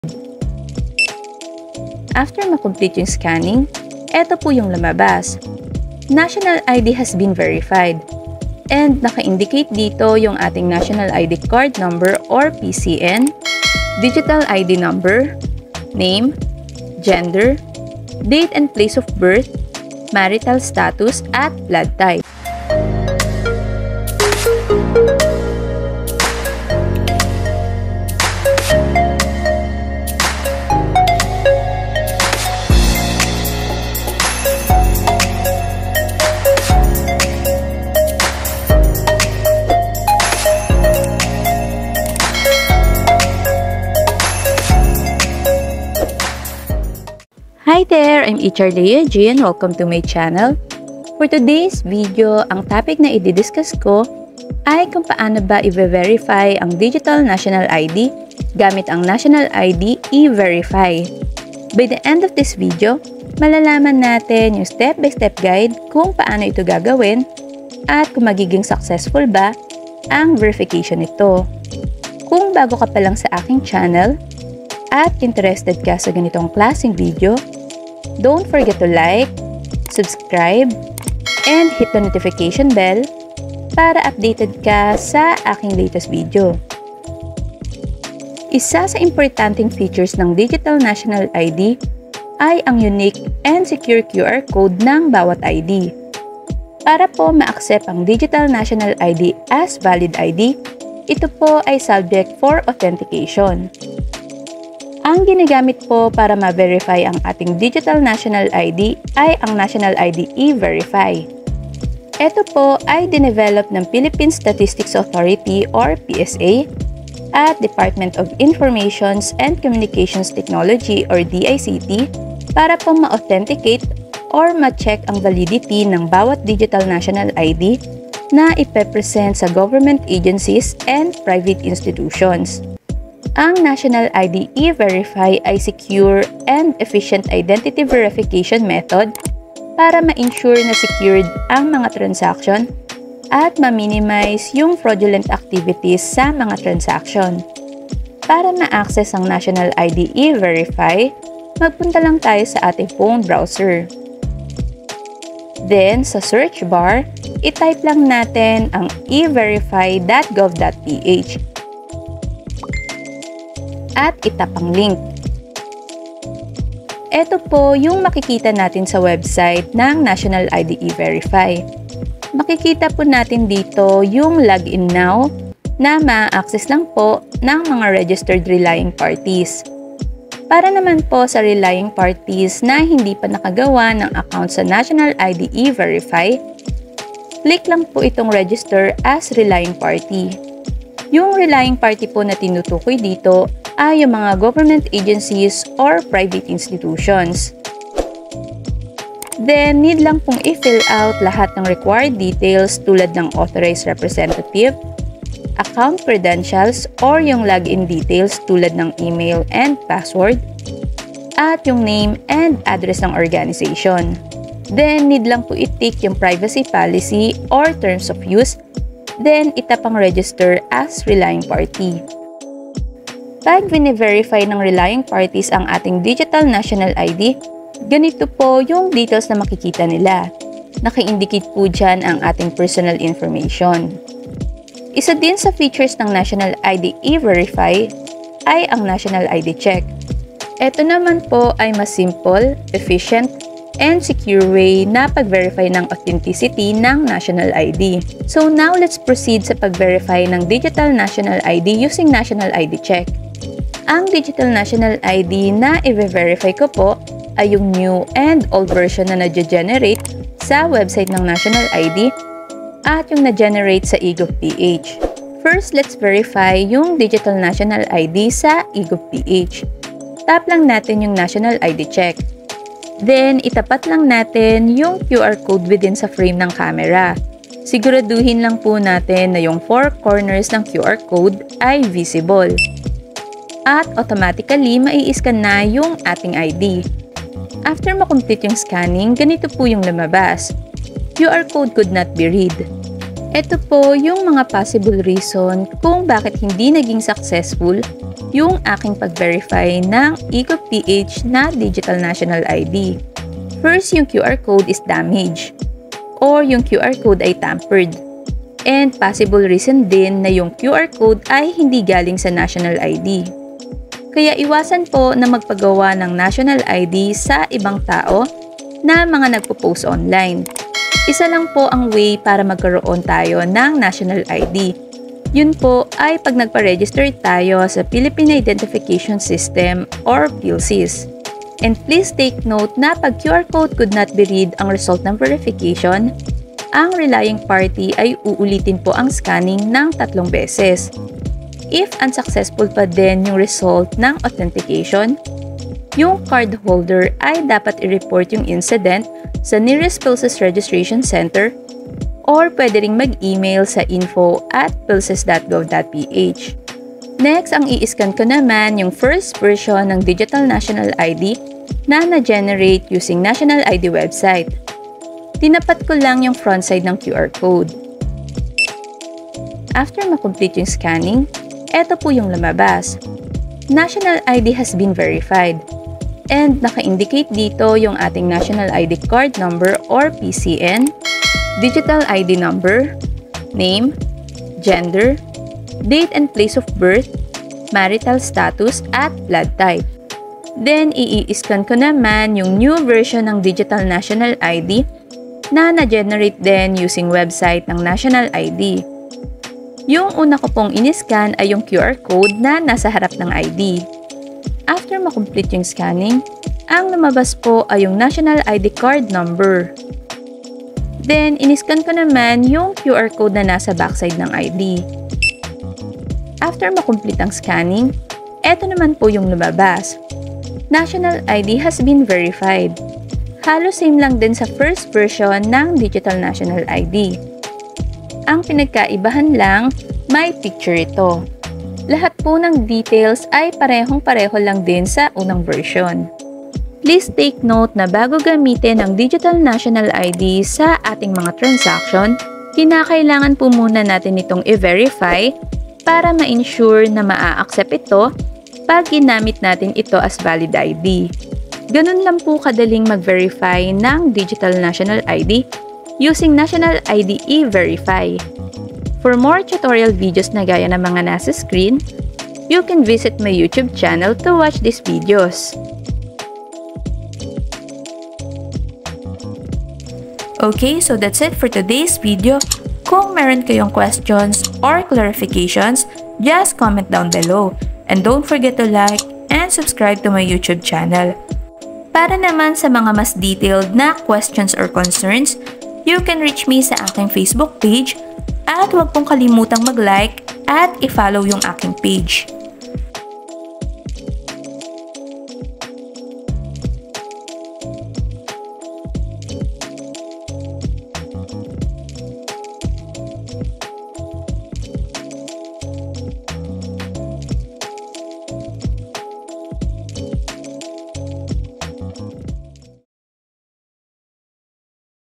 HOW TO VERIFY DIGITAL NATIONAL ID USING NATIONAL ID eVERIFY | HR LEAH G.
PAANO IVERIFY ANG DIGITAL NATIONAL ID

👉 Easy step by step guide
👉 Make sure na na verify na ang Digital National ID bago ito gamitin as valid ID